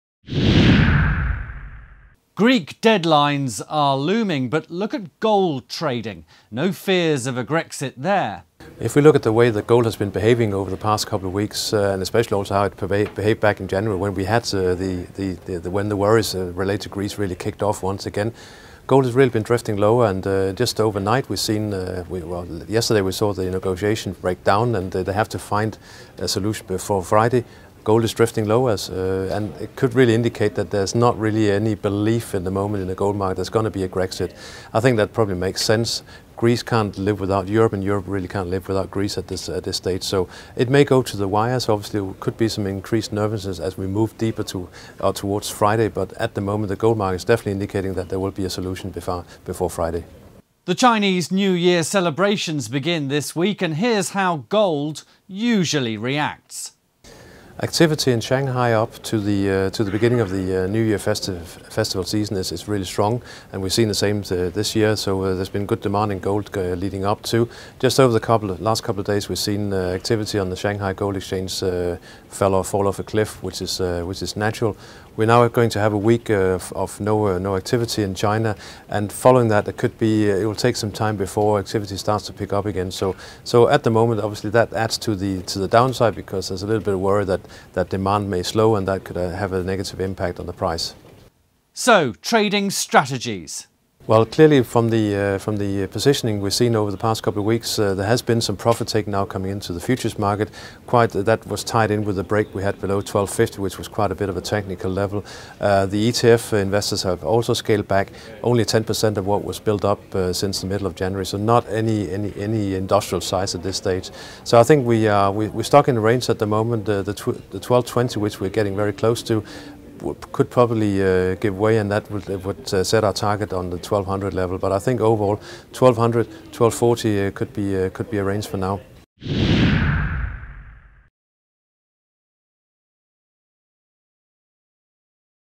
Grexit? A golden reason not to panic
Greek debt deadlines are looming and markets have been reacting but no sings of panic...yet. Saxo Bank's Head of Commodity Strategy Ole Hansen has been analysing the Gold market for clues and trading suggests a relaxed mood despite the most recent talks between the Greek finance minister and the Troika breaking down. So are Gold traders discounting a Grexit?

Gold is holding above USD 1219 with 1200 as the next key support level and 1240/50 the upside resistance levels, according to Ole who also explains what will happen to Gold prices when China begins week-long New Year celebrations this week.